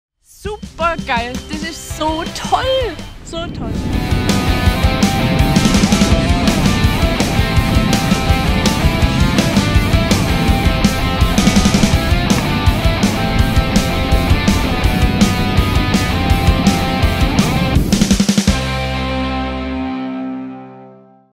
#7 Gleitschirm Tandemflug mit Nina
Wechsle jetzt Deine Perspektive und erlebe im Allgäu die faszinierendste, einfachste und purste Art zu fliegen!